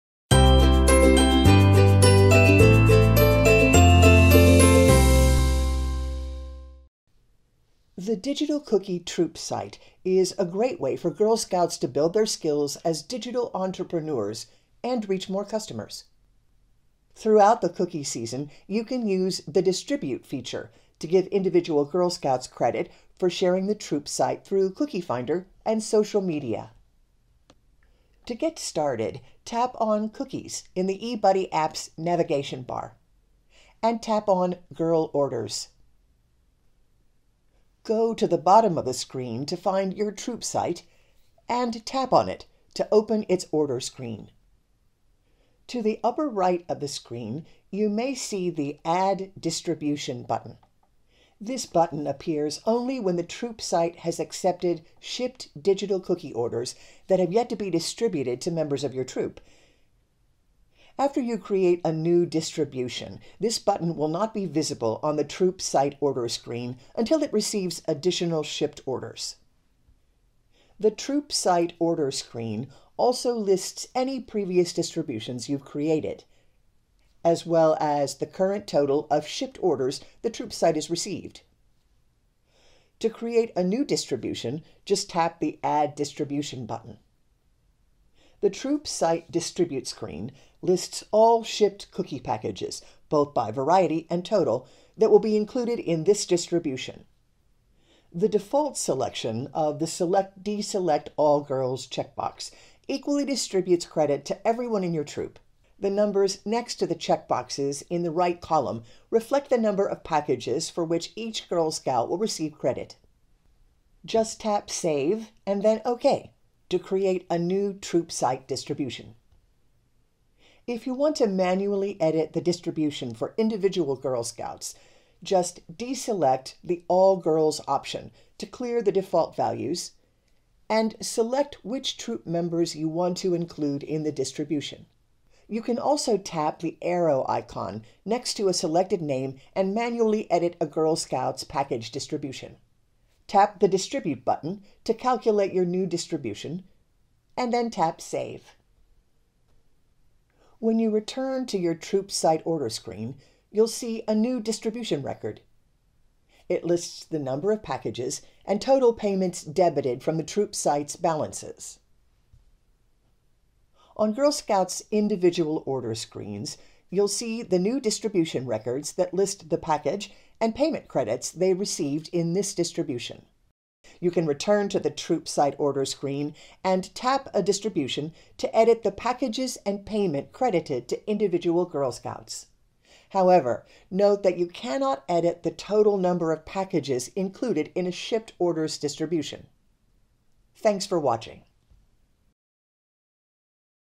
eBudde™ Volunteer Training: Distribute Troop Site Shipped Orders
Girl Scouts can share the Digital Cookie®️ Troop Site through Cookie Finder and social media, and you can use the Distribute feature to give them credit.

0:00 The Troop Site Distribute feature
0:27 Navigating to the Troop Site order screen
0:44 Troop Site distribution basics
1:22 Creating a new distribution
1:59 Editing distribution values
2:30 Girl Orders credit for Troop Site distributions